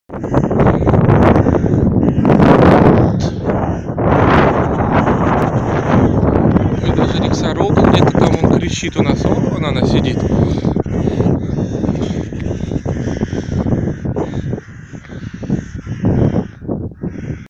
Грач Рики - орущие вороны (и даже одна сорока)
Орущие вороны в общем то стандартная практика при прогулках. При этом обычно орут ворОны и галки. Сегодня даже сорока стрекотала возмущенно. А вот грачи молчат.